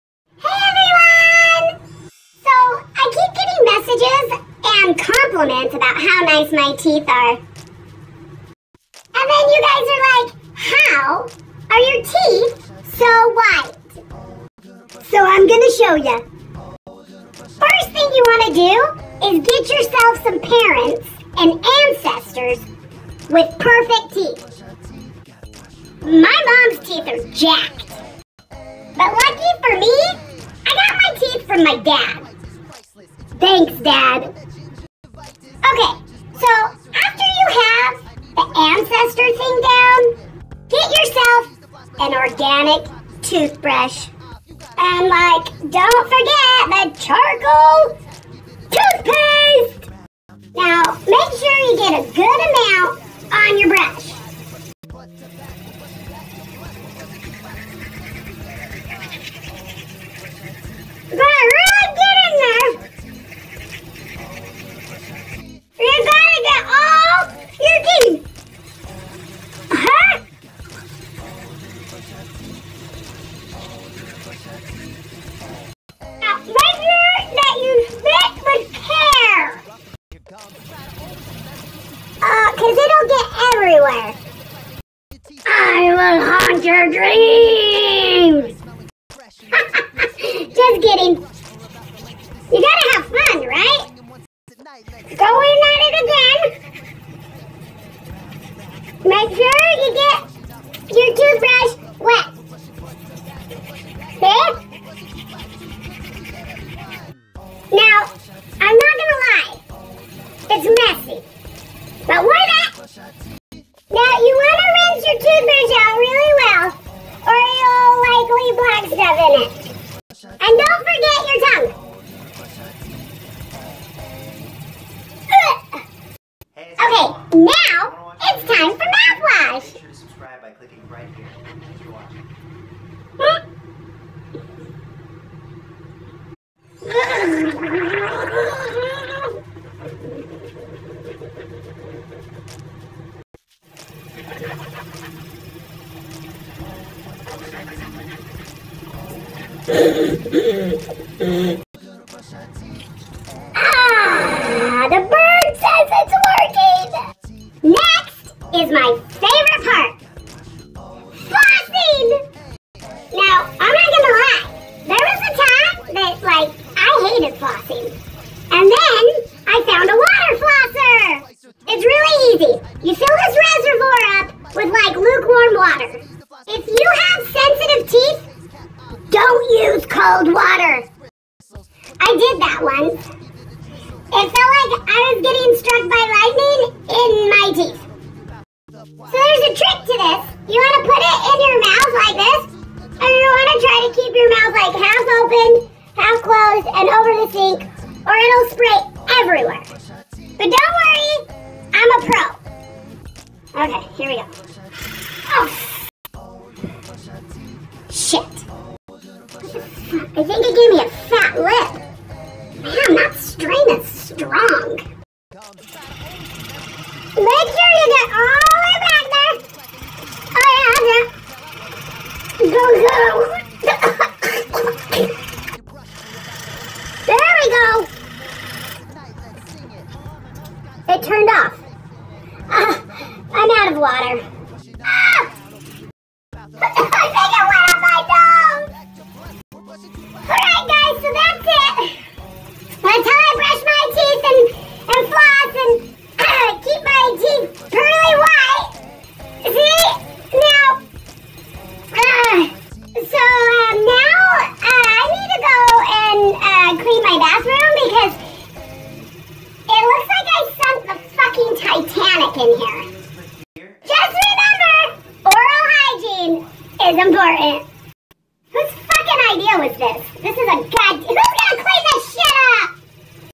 How I keep my teeth super white!
I get compliments and questions every day about my teeth and how they stay so white. So I thought I'd make a tutorial video for you. Enjoy.